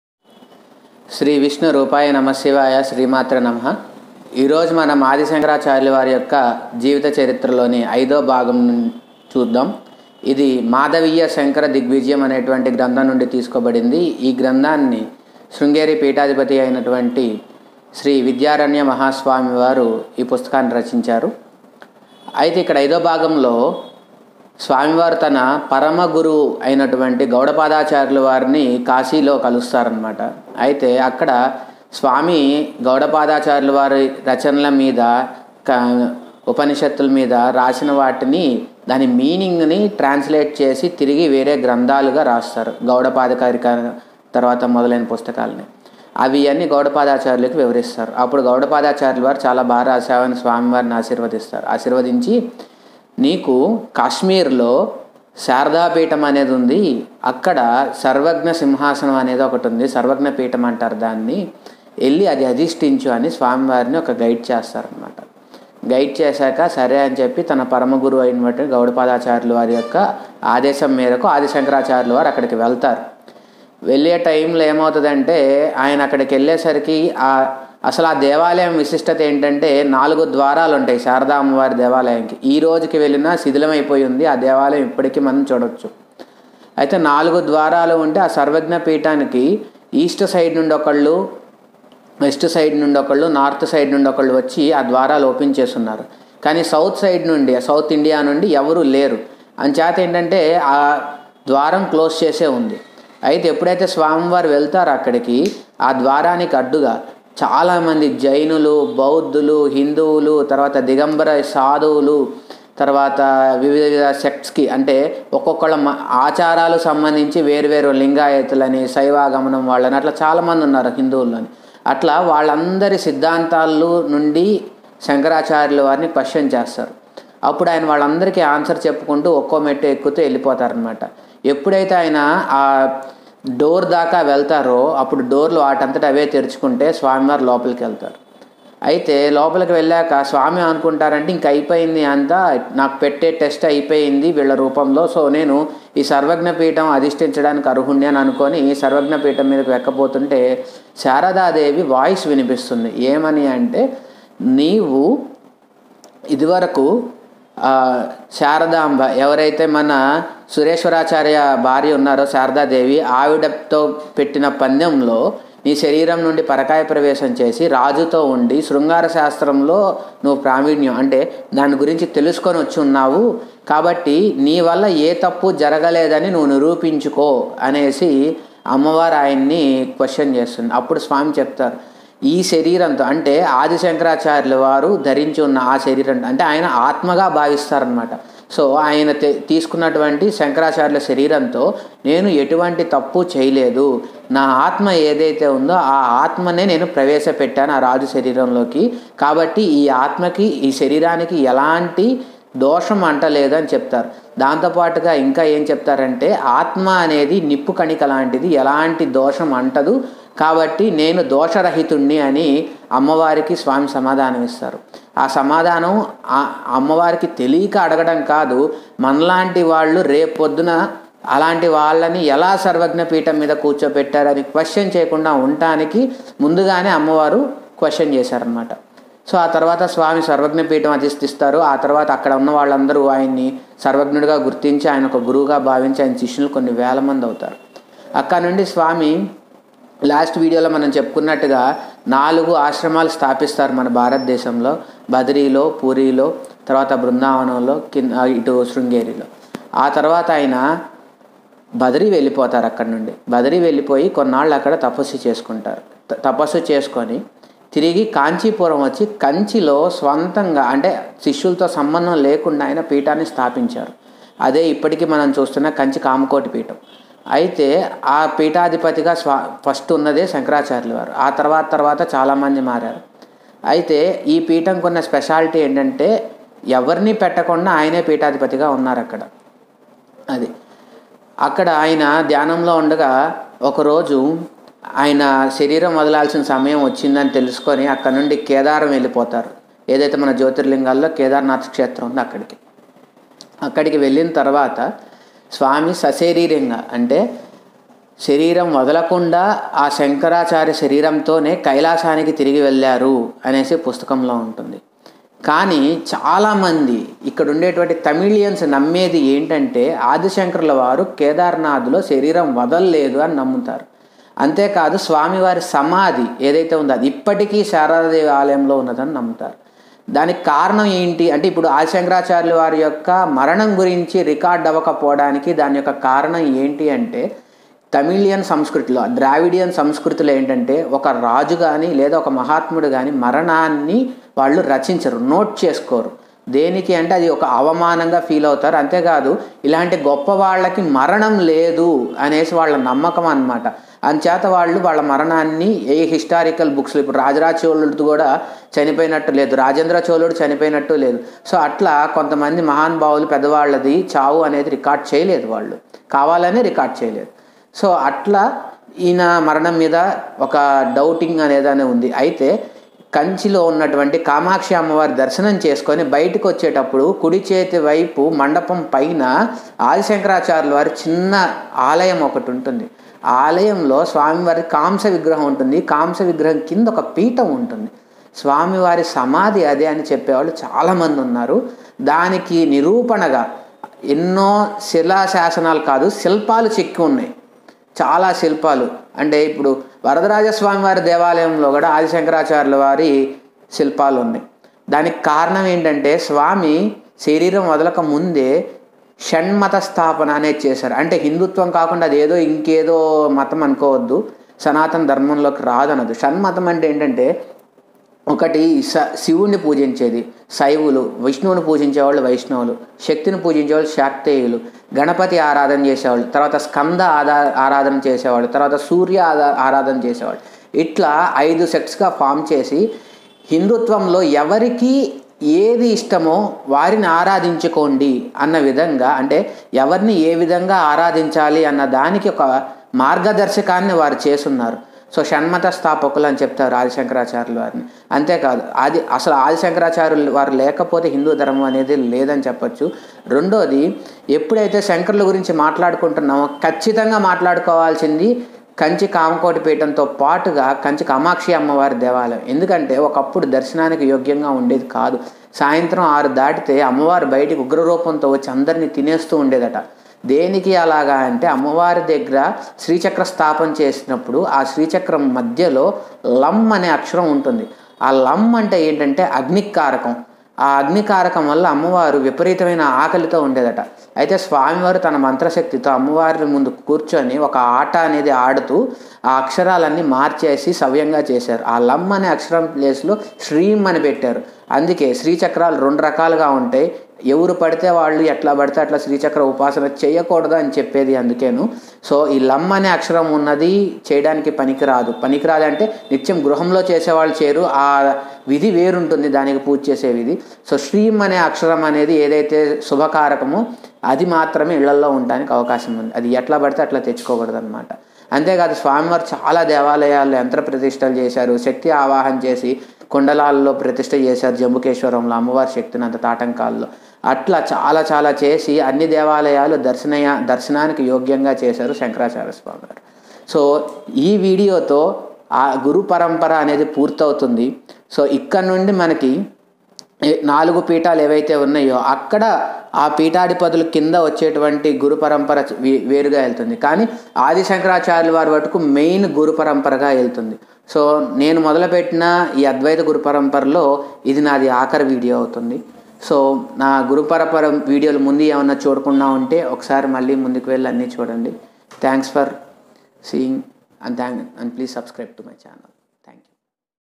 అద్వైత గురు పరంపర పార్ట్-9.5 ||శ్రీ జగద్గురు ఆదిశంకరాచార్య
శ్రీ జగద్గురు ఆది శంకరాచార్యుల వారి గురించి కంచి పన్నెండవ పీఠాధిపతి అయిన శ్రీ విద్యారణ్య మహాస్వామి వారిచే రచించిన మాధవీయ శంకర దిగ్విజయం అనే గ్రంథం నుండి ఆది శంకరాచార్యుల వారి యొక్క చరిత్రలోని ఐదవ భాగం (ఆఖరి భాగం) మీ కోసం